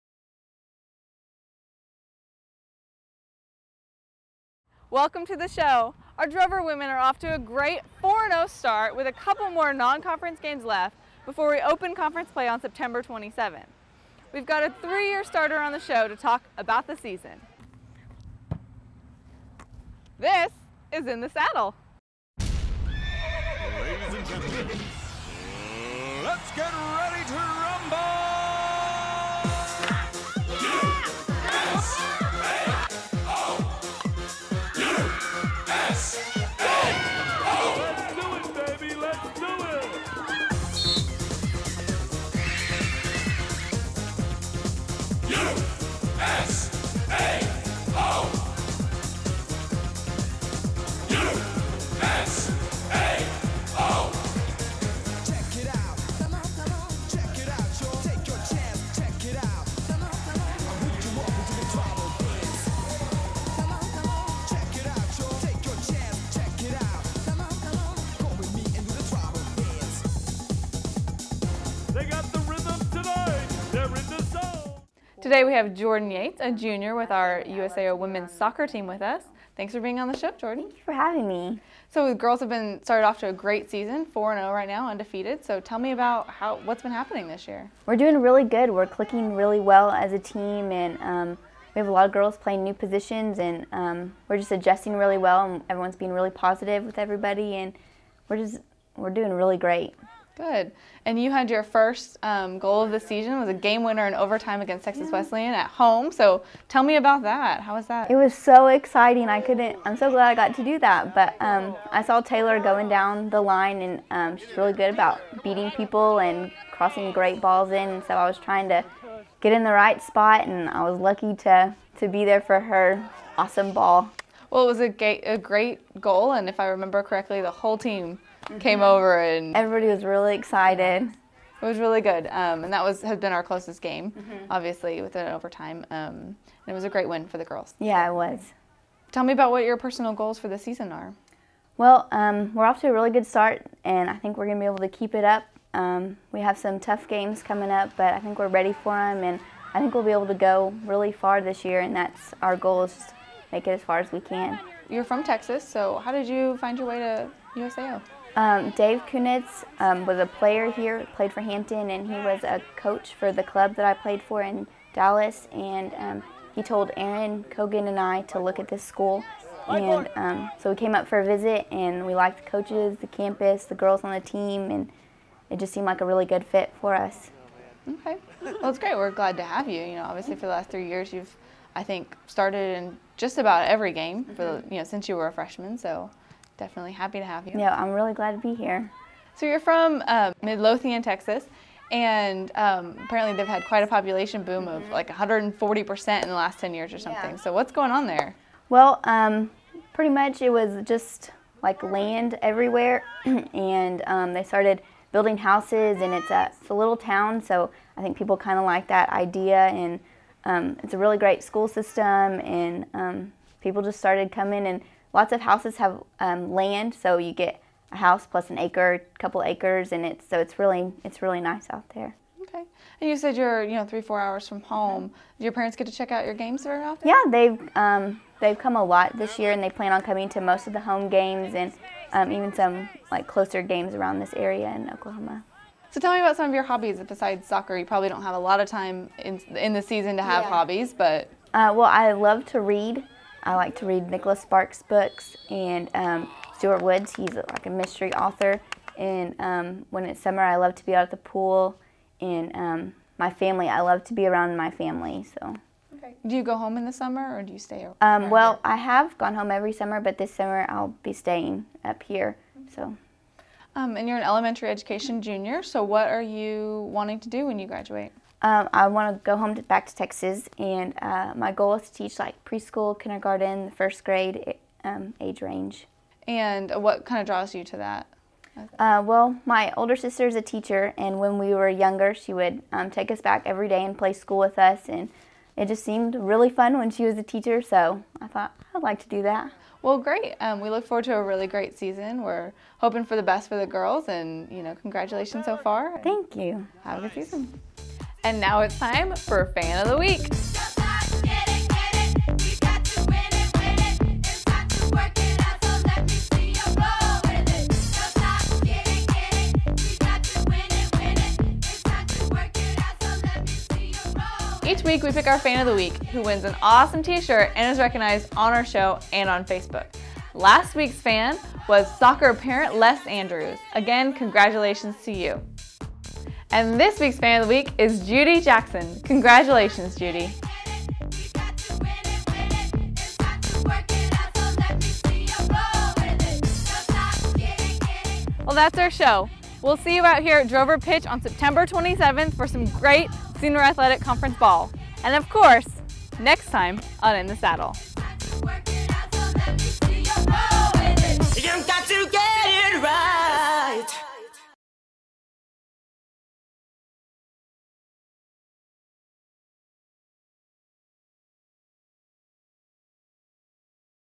In the Saddle — Jordyn Yates
CHICKASHA -- Tuesday night, the Drover women knocked in another win, giving them one of the best starts in school history, and junior Jordyn Yates said it just comes down to the team chemistry.

"We're doing really good. We're clicking really well," she said. "We have a lot of girls playing new positions and we're just adjusting really well."

Yates scored her first goal of the season in overtime against Texas Wesleyan last week, but Yates credits teammate Taylor Fain, who set her up with an assist.

"It was so exciting. I'm so glad I got to do that," she said. "I was trying to get in the right spot, and I was lucky to be there for her awesome ball."

When she graduates, Yates said she hopes to become an elementary school teacher, focusing on the younger grades.

"My older sister is teacher and when we were younger she'd take us back everyday and play school with us," she remembers. "It just seemed really fun when she was a teacher, so I thought I'd like to do that."